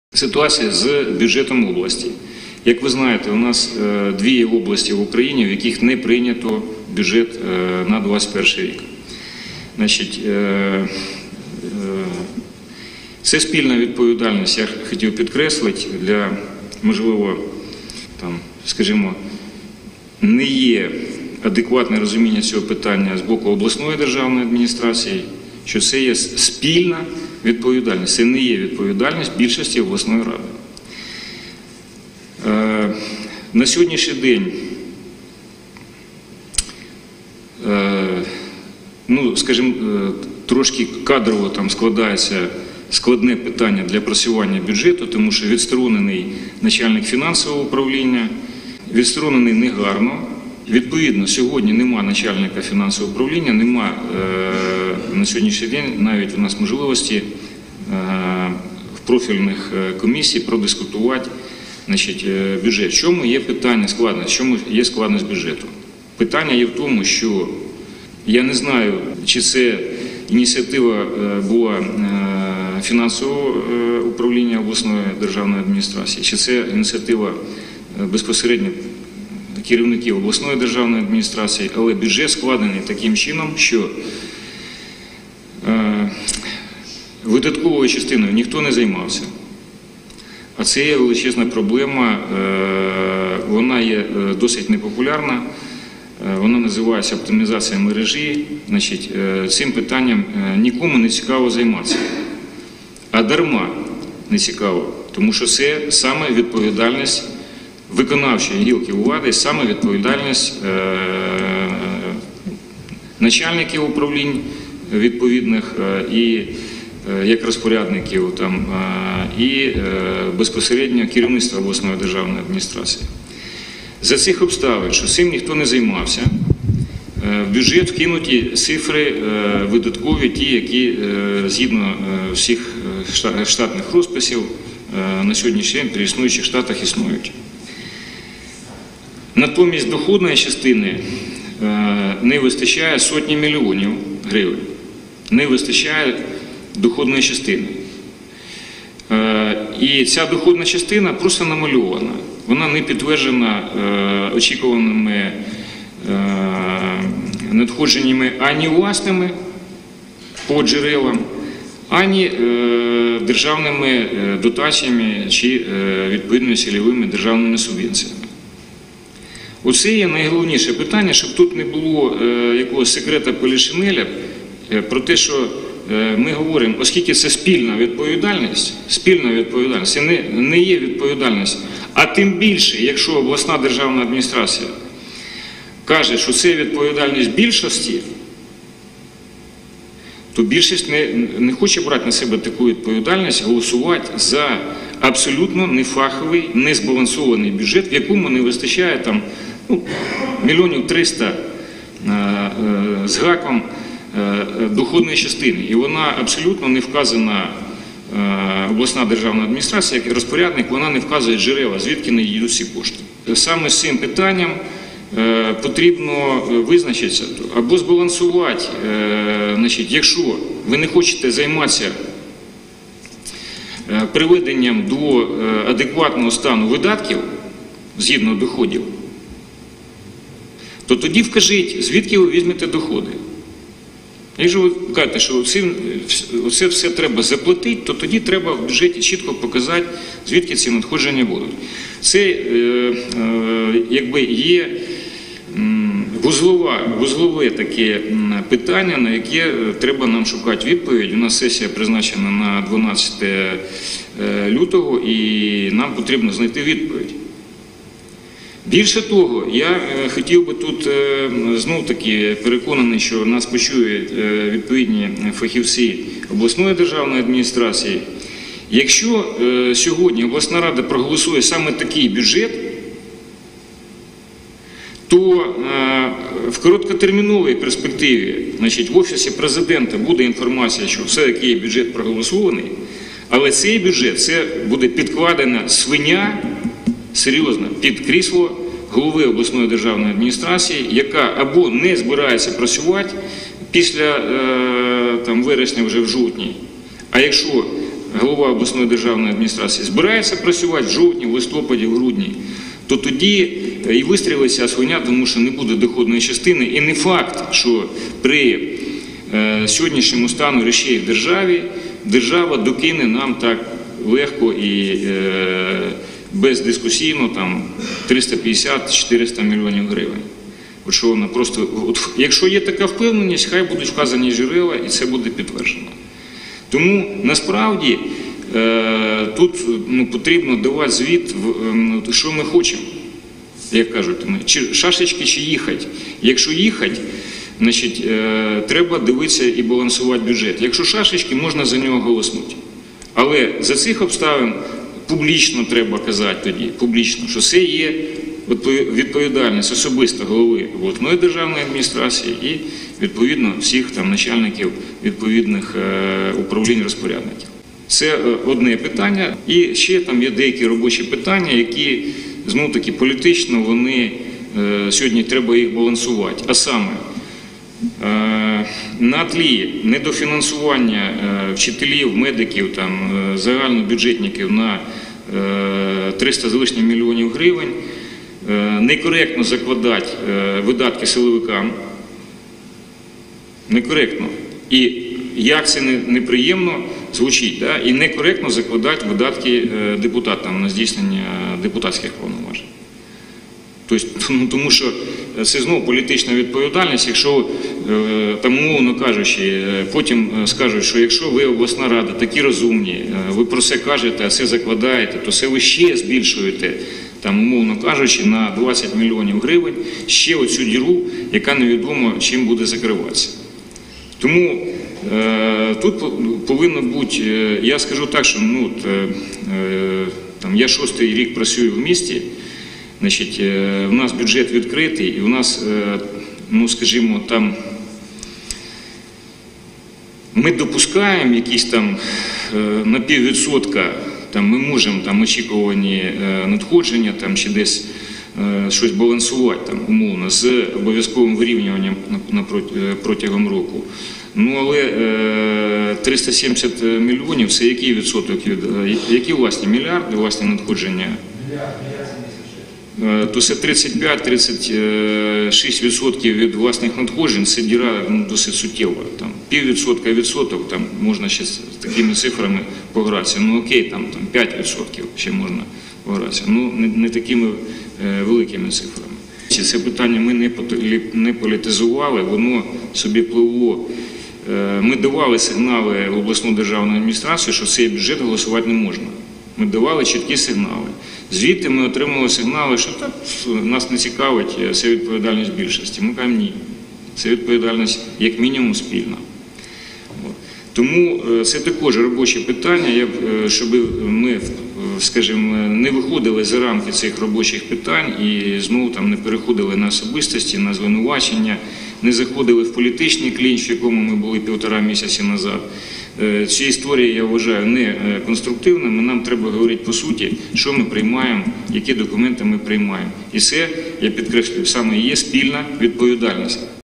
Атрошенко про прийняття проєкту бюджету області
Атрошенко про прийняття проєкту бюджету області та спільну відповідальність депутатської більшості та керівництва ОДА